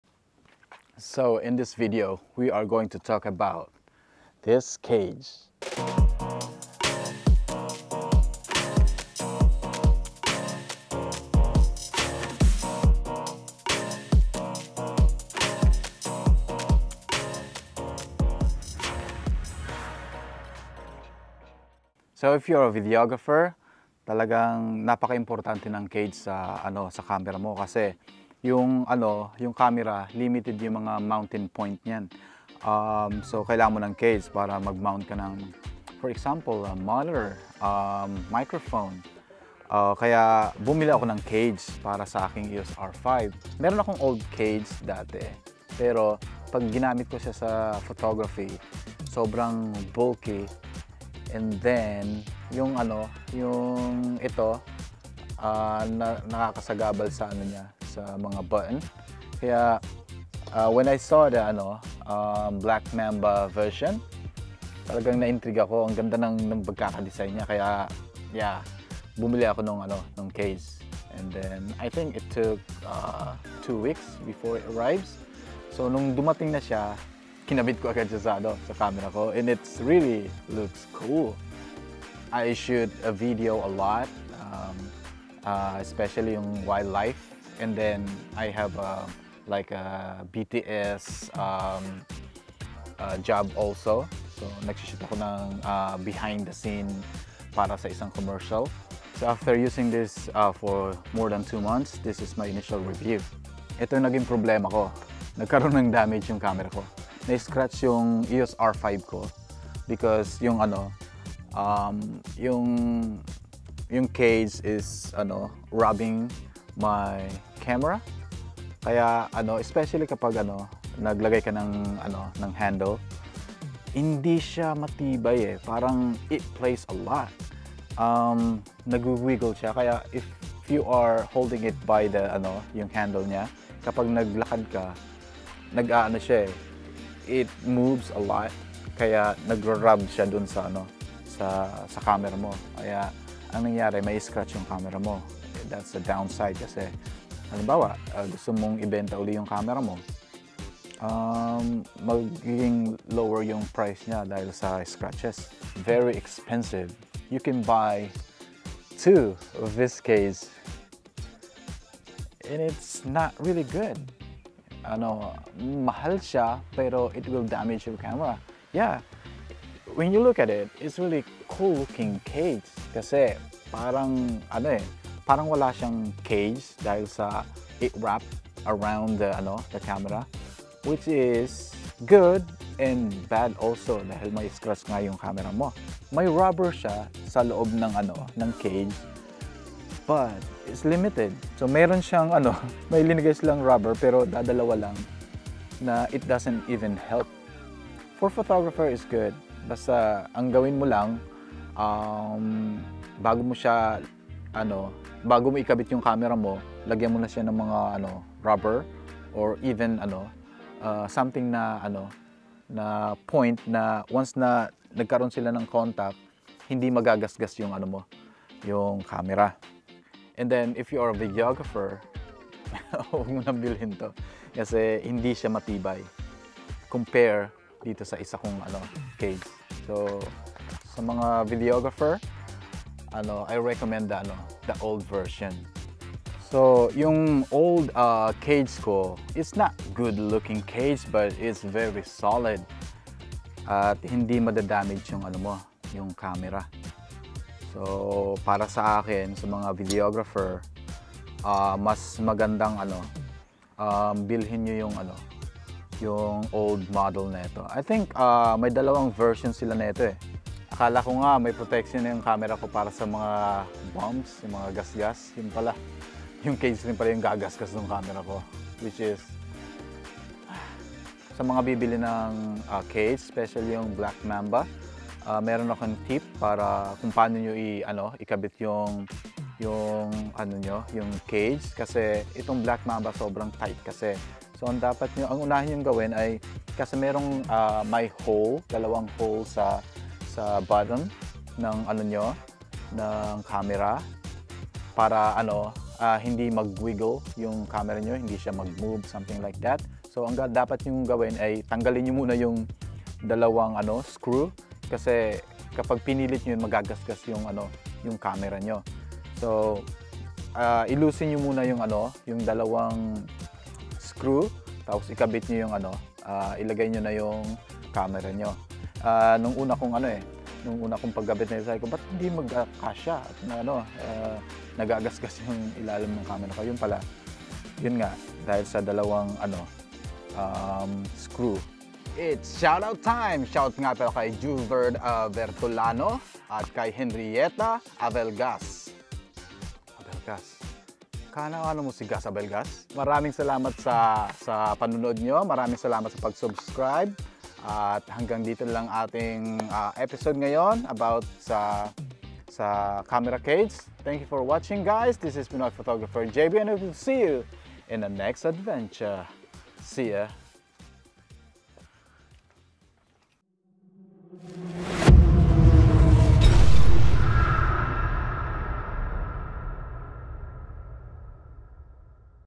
smallrig, black mamba review, the good and the bad, eos R5/R6 ( tagalog )
this is my short review of the smallrig black mamba cage